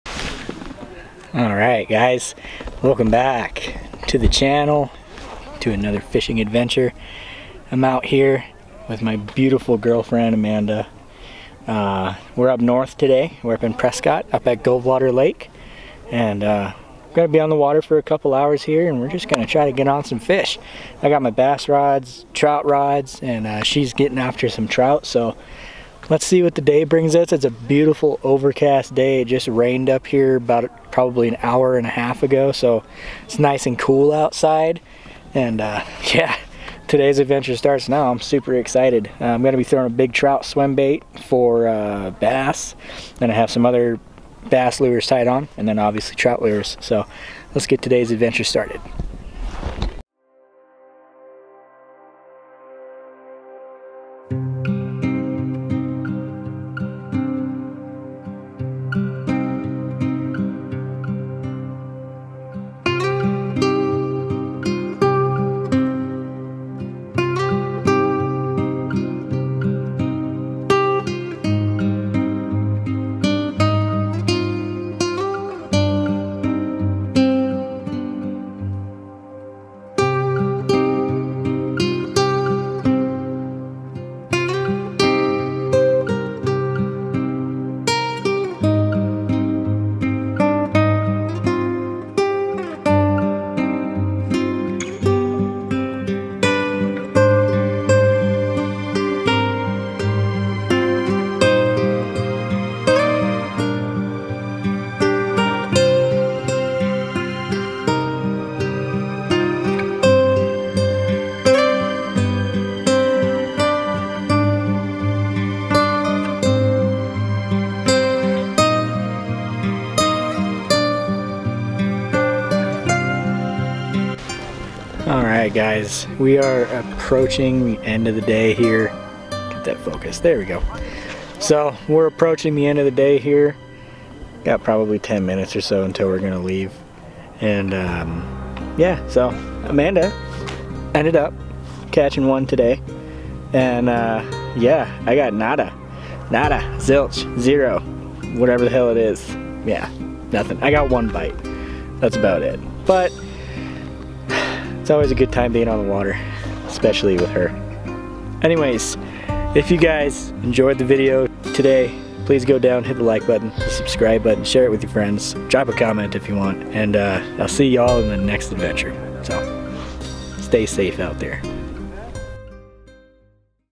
Chasing High Country Arizona Trout | Arizona Trout Fishing | Goldwater Lake Az
Hey folks, I hope you all enjoyed todays adventure! I kept this one short and sweet for the sheer fact of only 1 trout being hooked into! Thats why they call it fishing and not catching! Until next time!